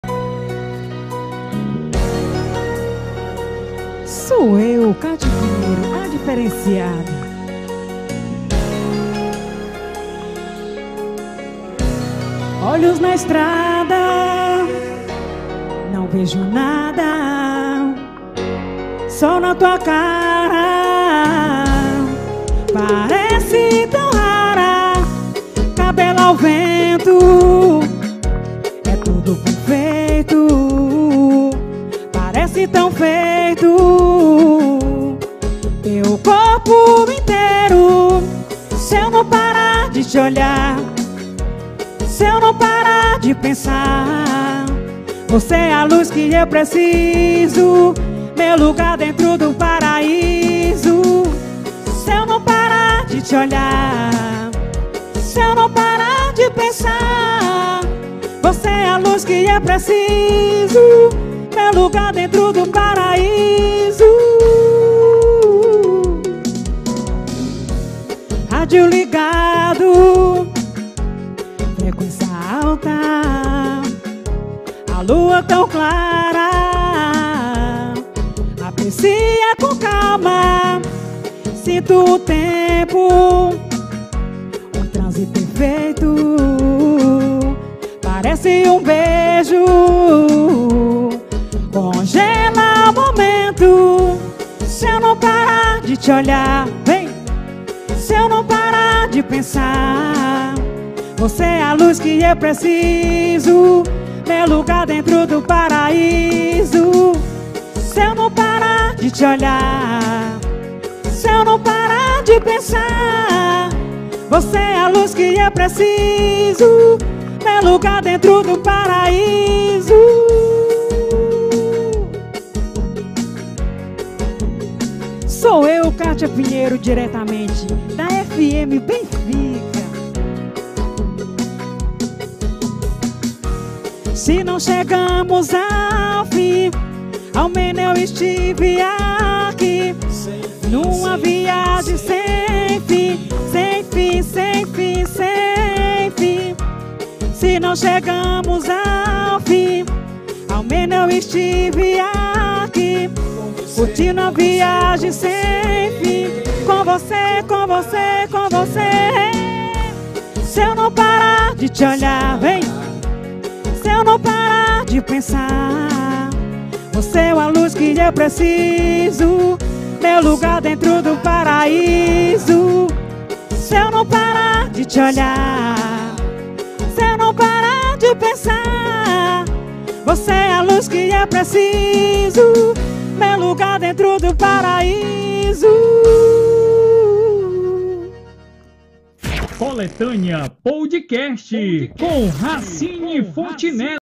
Coletânea PodCast - Kátia Pinheiro, a diferenciada
Não tem como ficar parado ao som de @katia_a_diferenciada no @coletanea_podcast !